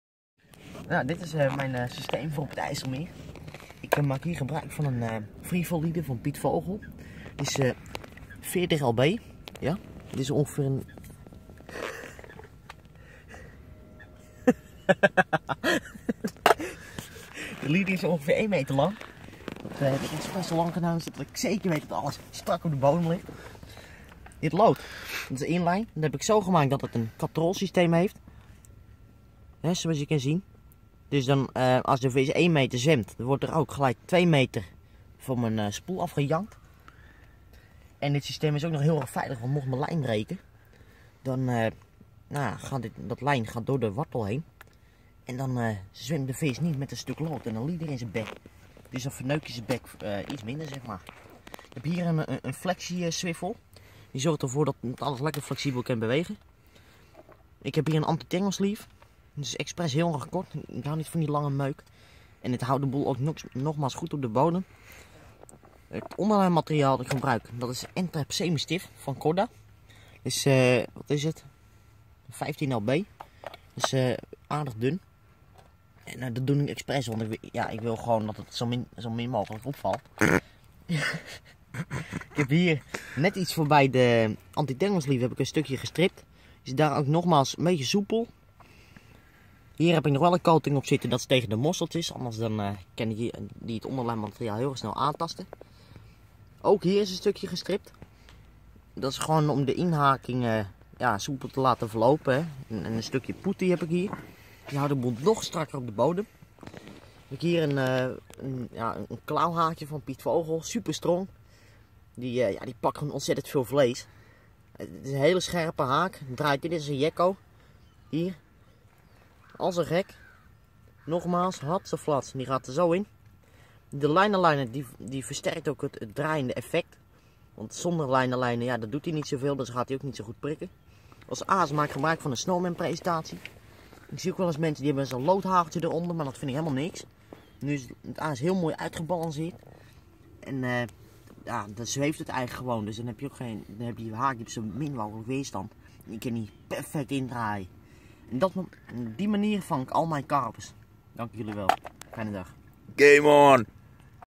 Karpervissen met Robin op het ijsselmeer
Uitleg van de laatste meter